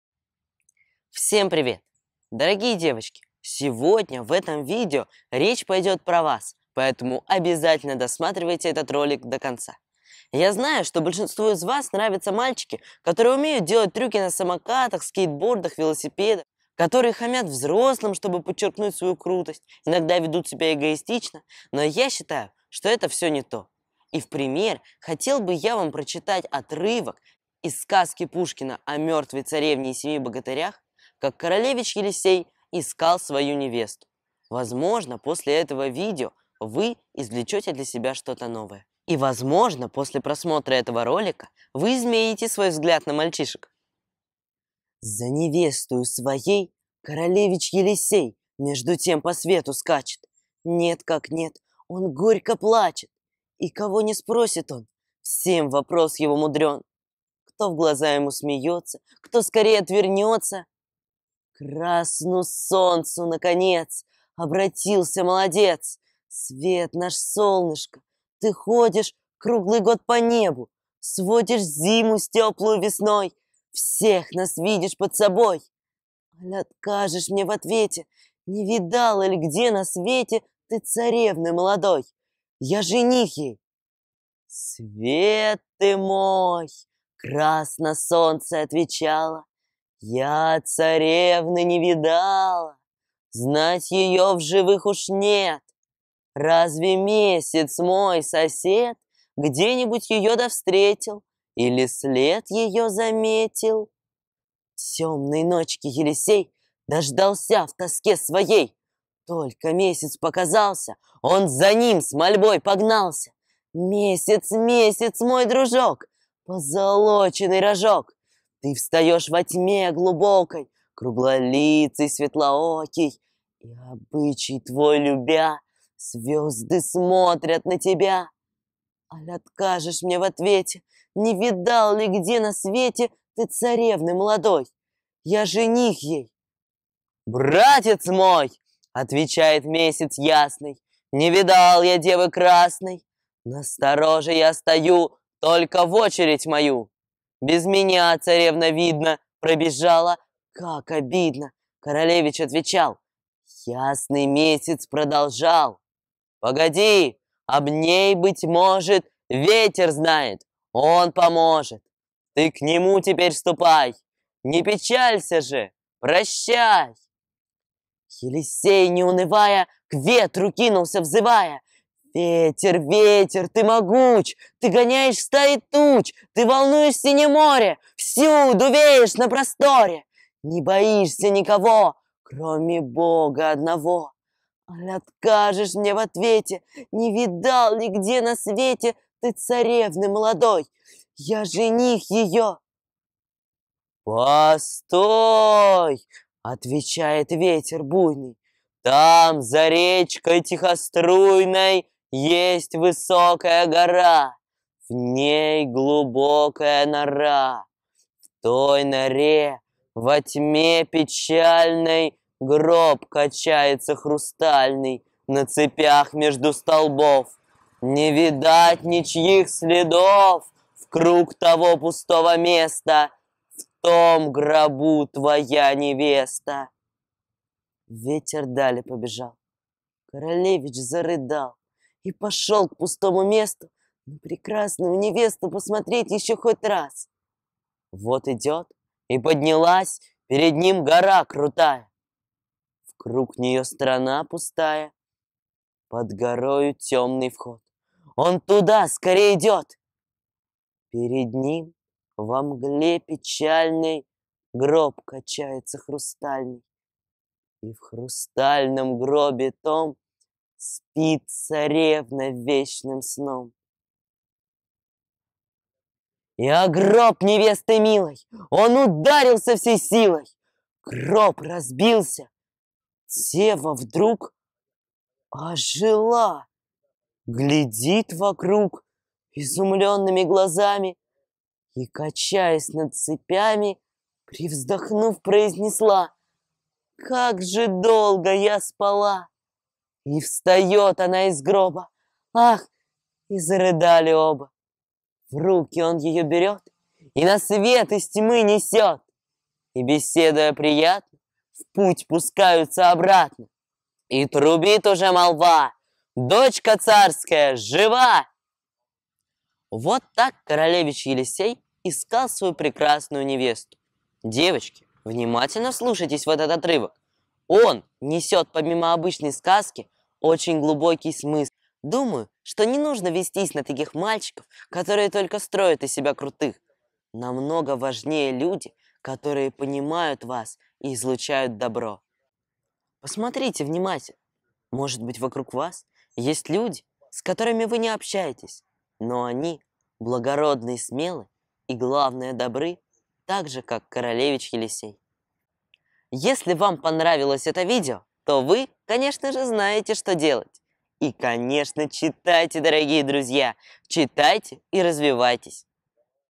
Советы для подростков. Девчонки ваш выбор? Пушкин стихи.
Всем привет! В этом видео я дам небольшие советы для подростков девочек и  прочитаю отрывок из сказки "О мёртвой царевне и семи богатырях" в котором королевич Елисей искал свою невесту.
Этот отрывок вроде - бы все знают, и в тоже время многие не догадаются сколько смысла в нём скрыто. Только сейчас когда я углубился в его изучение, мне стало понятно какой благородный и мужественный был королевич Елисей.

Ну, а смысл необычного название заключается в том, чтобы узнать у девчонок, каких мальчиков они выберут, таких благородных как королевич Елисей, или крутых, но грубых парней?  Выбор за вами девочки ! Пушкин стихи.